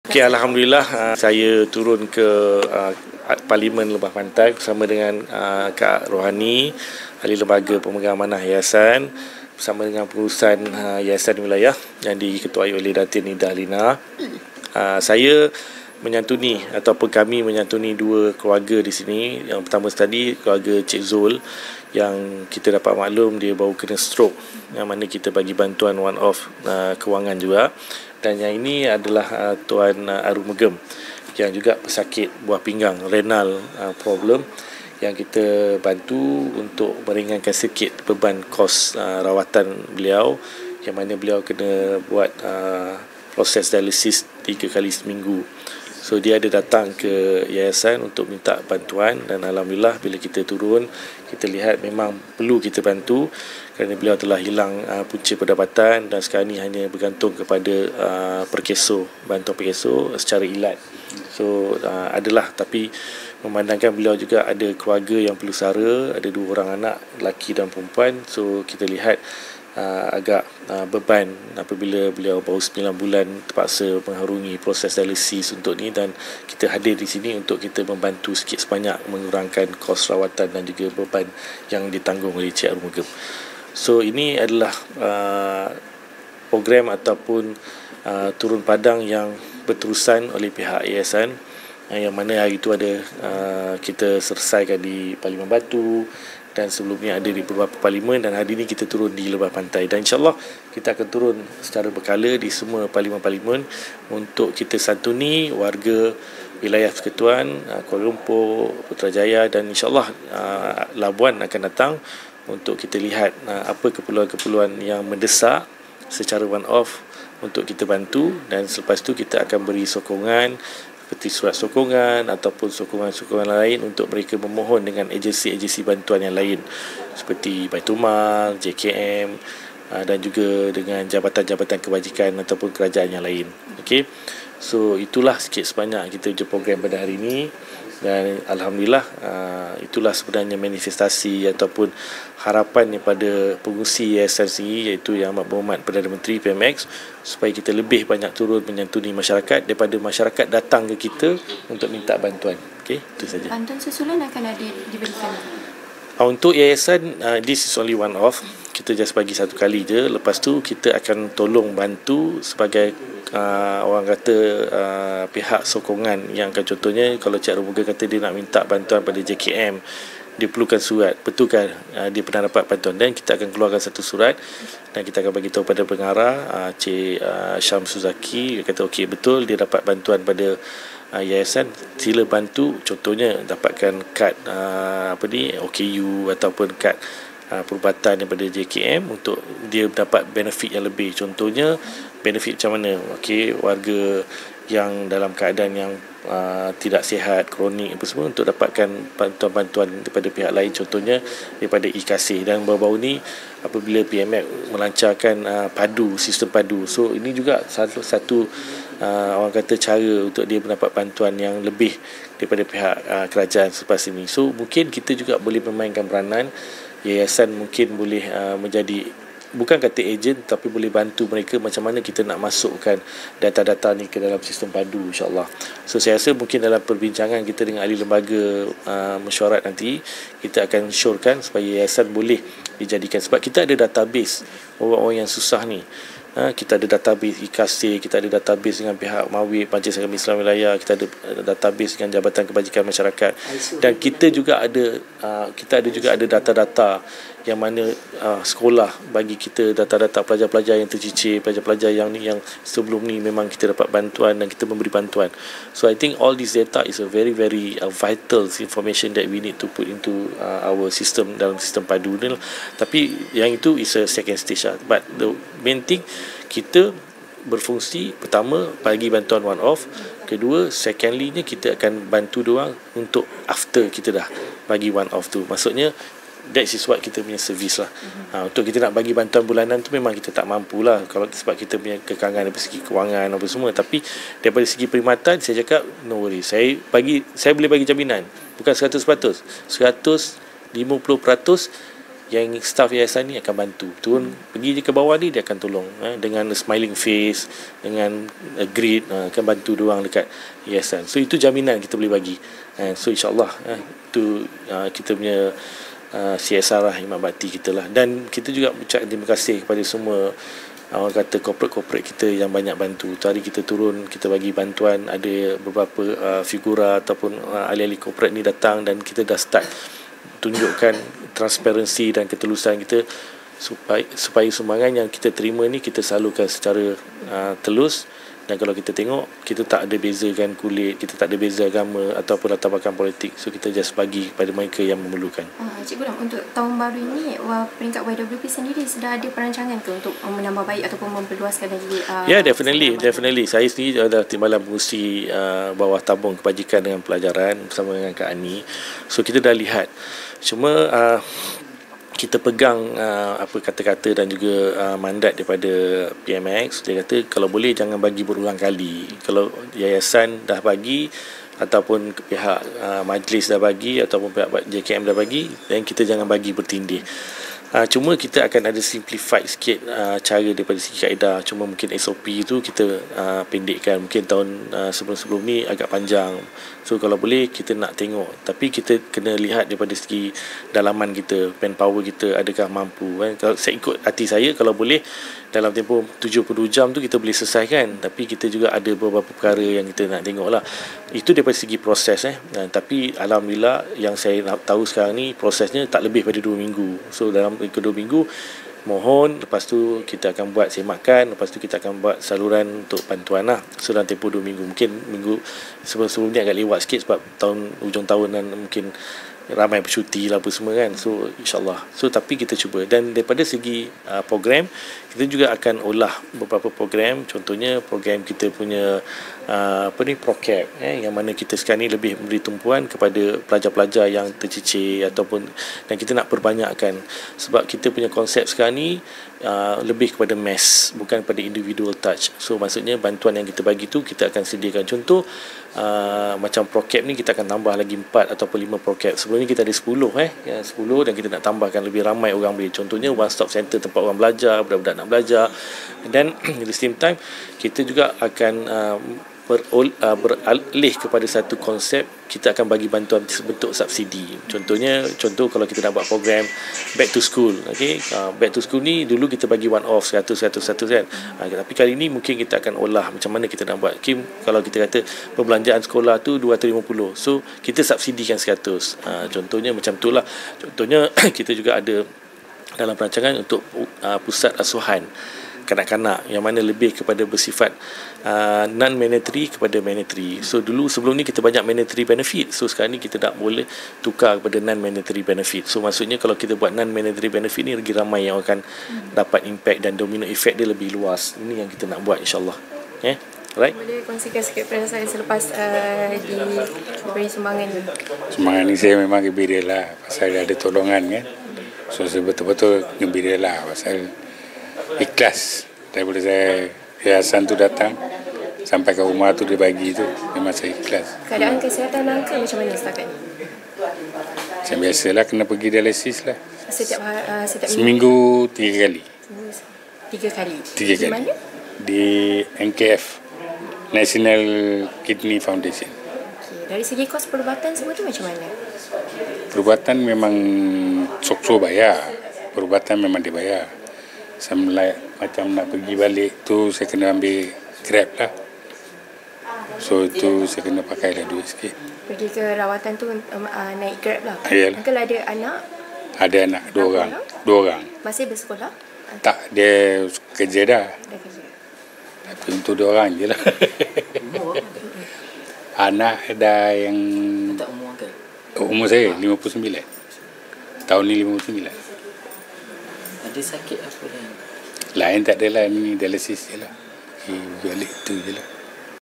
4 Jan 2024 - Majlis Penyerahan Sumbangan Bantuan Khas YWP kepada 2 orang penerima
04 Januari 2024:

• Majlis Penyerahan Sumbangan Bantuan Khas Yayasan Wilayah Persekutuan (YWP) yang disampaikan oleh Ahli Pemegang Amanah YWP, Tuan Gulam Muszaffar Gulam Mustakim kepada 2 penerima.

• Penerima 1 : Zulkapli b Saleh
Perincian Kes :
- Pemohon merupakan warga emas yang mengidap penyakit kencing manis dan stroke sebelah kiri.
- Isterinya juga mengidap penyakit kencing manis & Kolestrol.
- Pendapatan hanya menerima pencen tentera RM1,400/ bulan.
- Mohon bantuan perubatan bagi menampung kos ubat-ubatan.

• Penerima 2 : M. Arumugam a/l Mariappan
Perincian Kes :
- Pemohon mengidap penyakit kencing manis & buah pinggang (Hemodialisis 3 kali seminggu), darah tinggi dan stroke sebelah kiri.
- Isteri juga mengidap penyakit kencing manis.
- Pendapatan diterima dari Socso RM1,100/bulan.
- Memerlukan bantuan kewangan bagi menampung kos perubatannya.